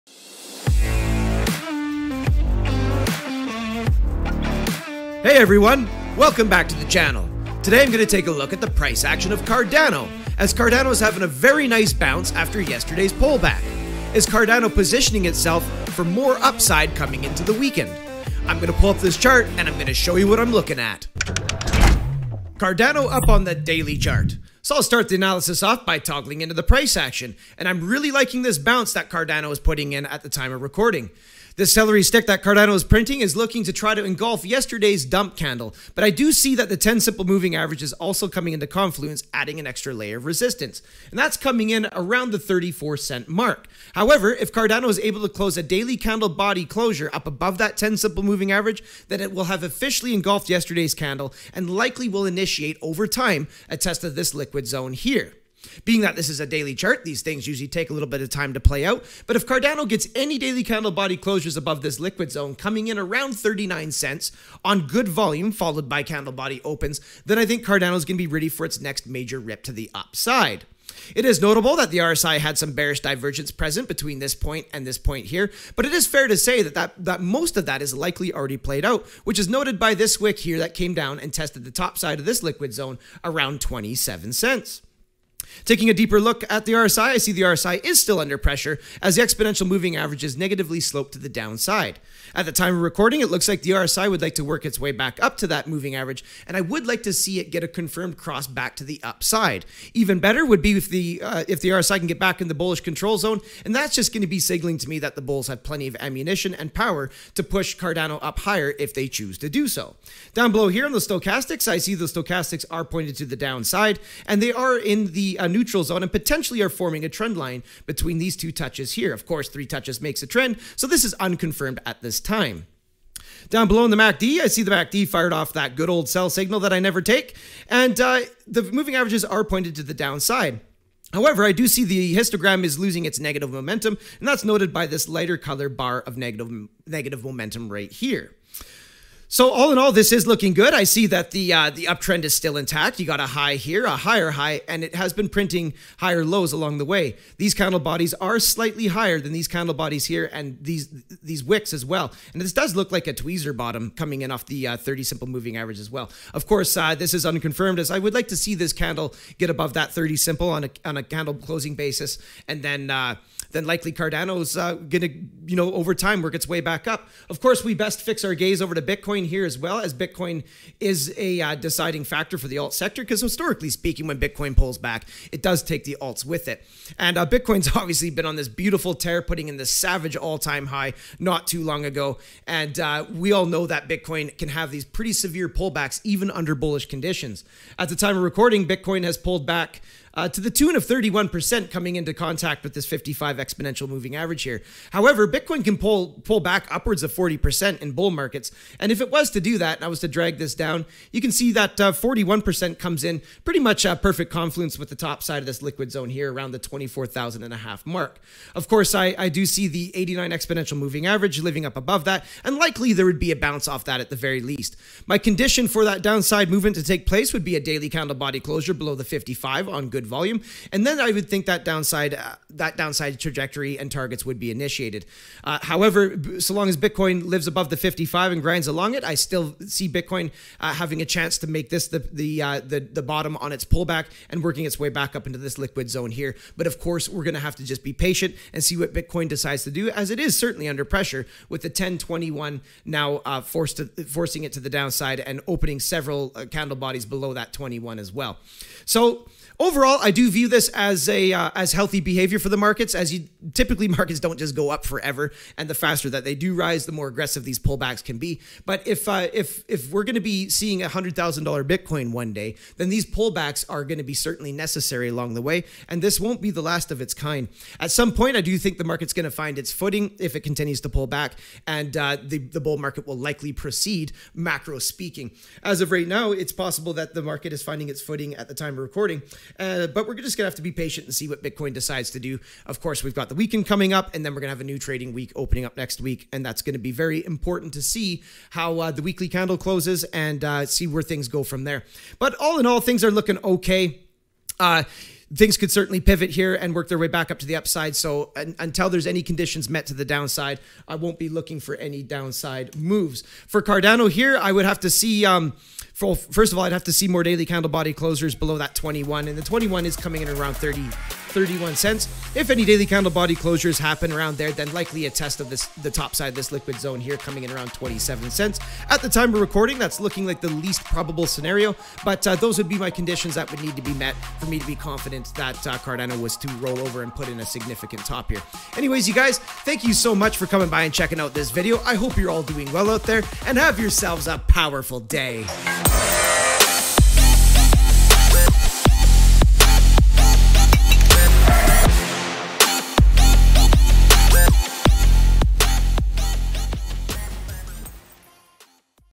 CARDANO - PRICE ANALYSIS - IS THE PULLBACK OVER!?
Time for a Cardano price analysis! Is the pullback over for Cardano? Cardano seems to be having a really nice bounce! Kiel takes a look at the Cardano chart to get an idea as to where Cardano could be heading next!